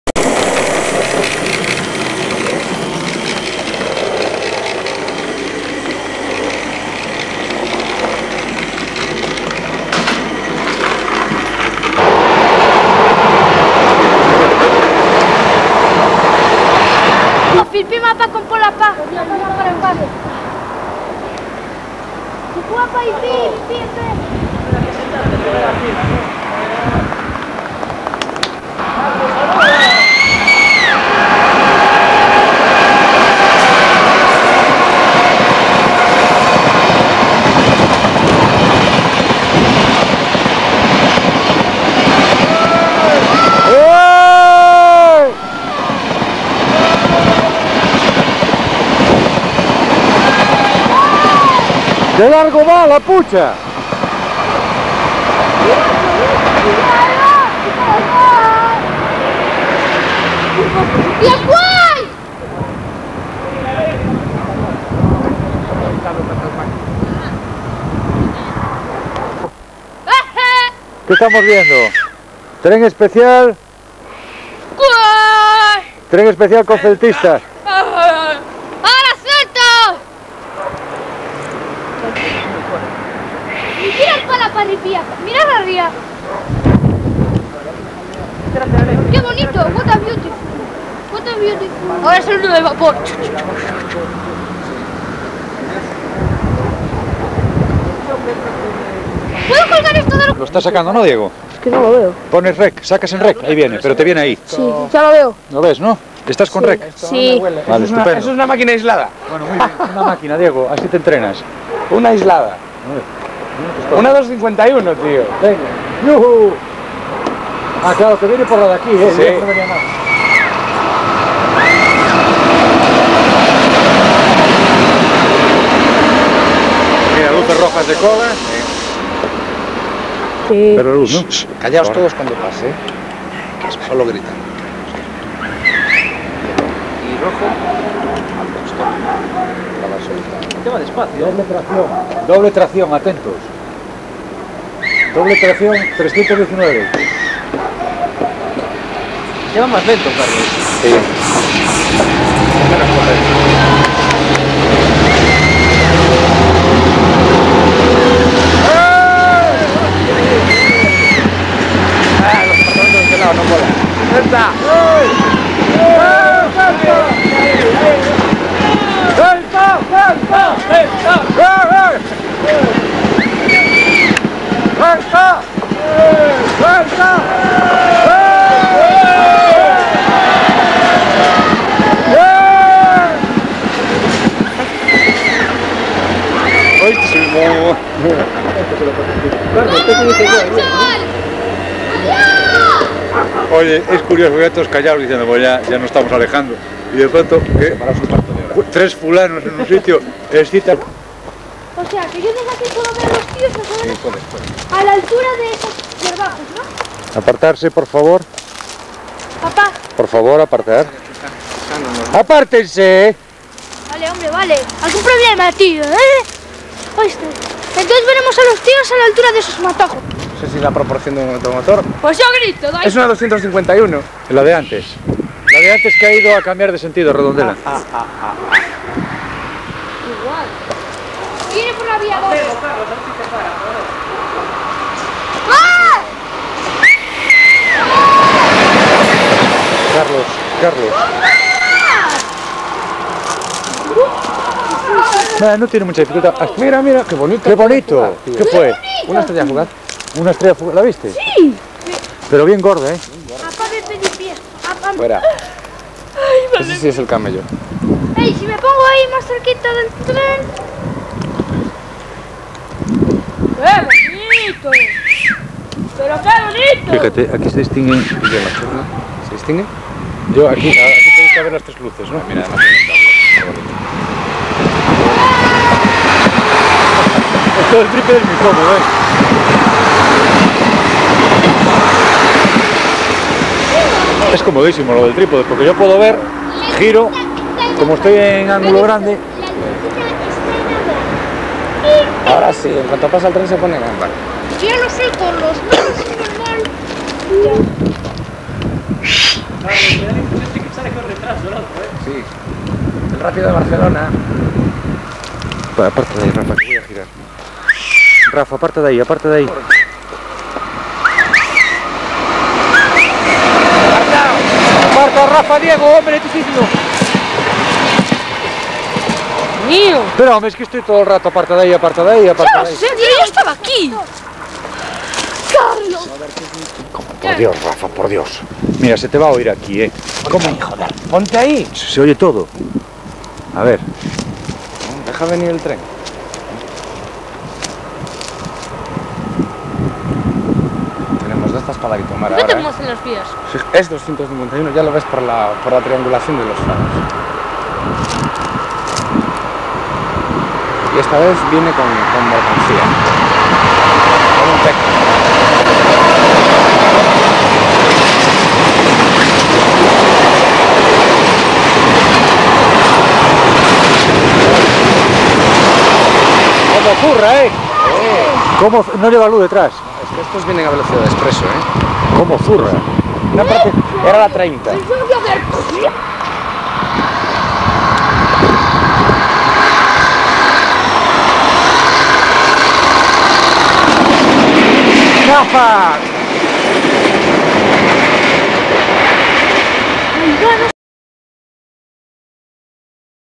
Especiales CELTA
En 1994 final de la Copa  del rey, a pie de via en Rande filmamos a los dos largos trenes especiales destino Madrid